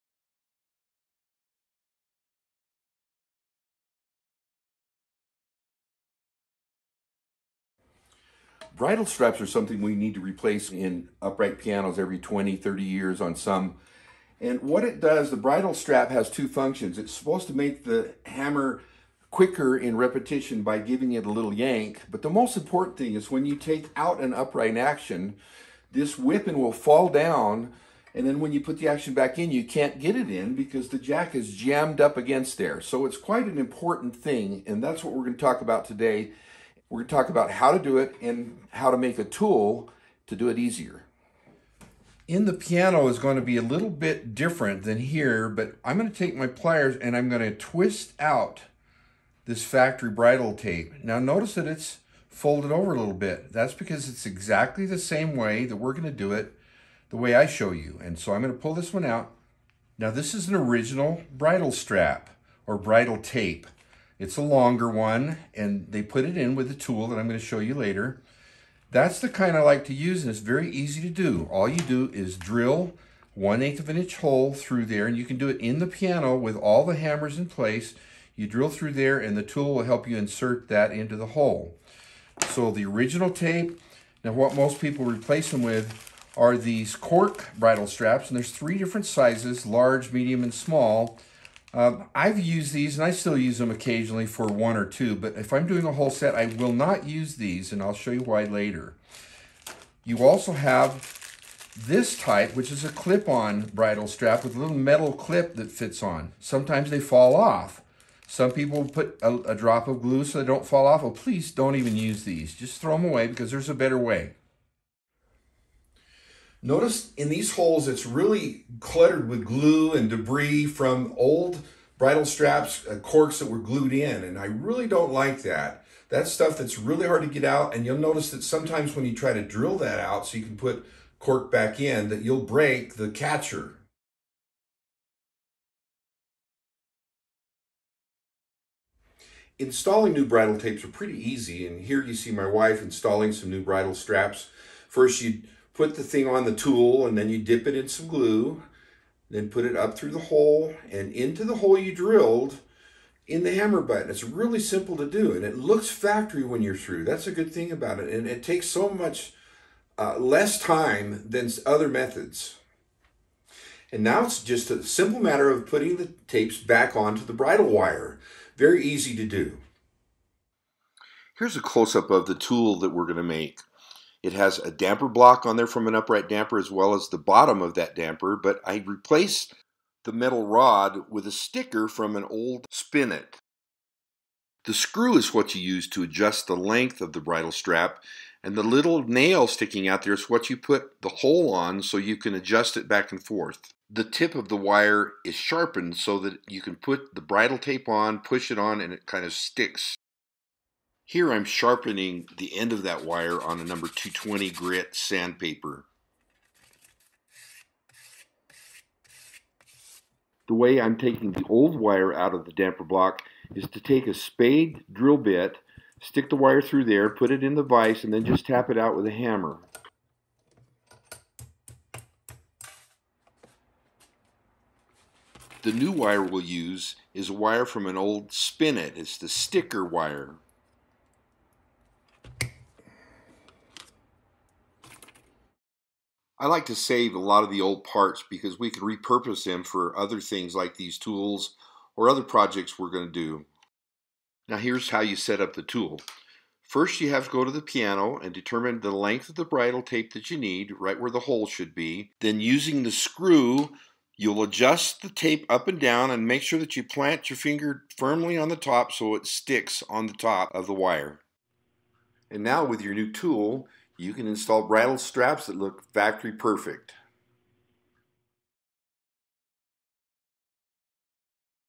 Pro Tip 2: Bridle Strap Inserter for Upright Pianos
Bridle straps, or bridle tapes on upright pianos, occasionally need replacing. This video features a simple, homemade tool, which is used to insert the bridle strap into the hammer butt, executing a job that looks factory perfect! Cork bridle straps may be used, but cork can become brittle in time and may break. Gluing in the cork straps can make future work very difficult, as the video shows. The video also shows how to make the tool and how easy it is to use. Find more information and see my eBooks at PTTBooks.com